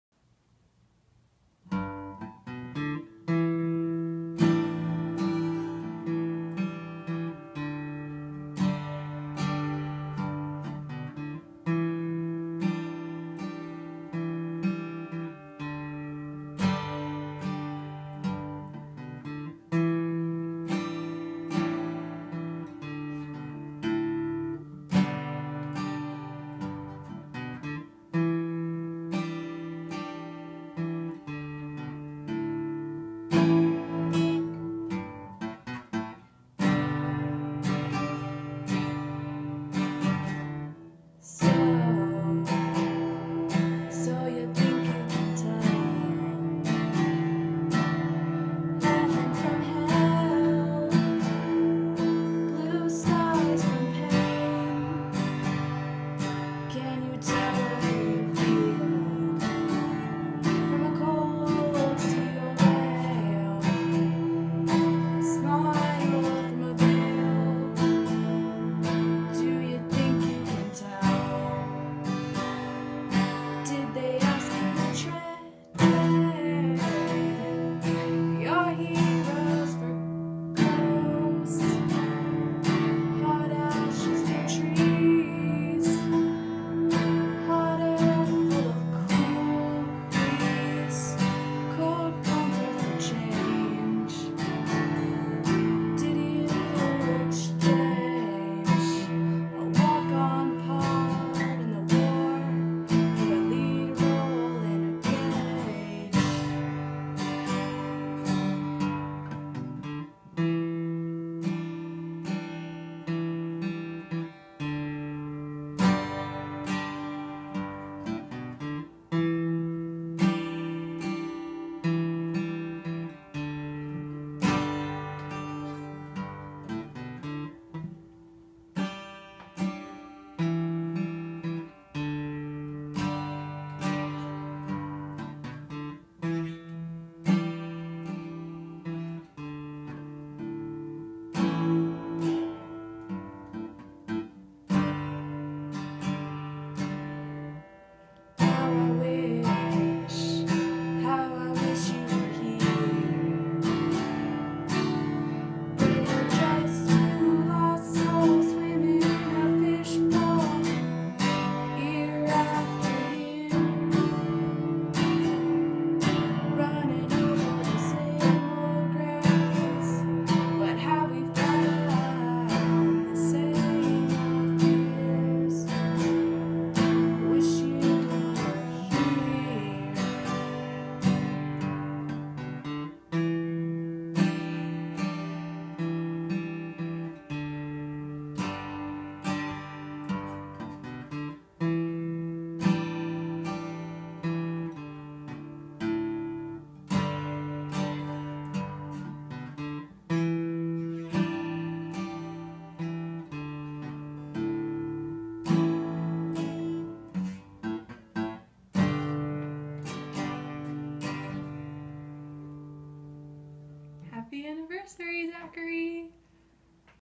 Happy Anniversary Beby!
Okay so I put this together last minute and I make A LOT of mistakes (singing and the guitar are really not up to par lol) but I really wanted to play it for you anyways :3 i love you so much, these past two years have been nothing but life changing. I hope you enjoy this version of our song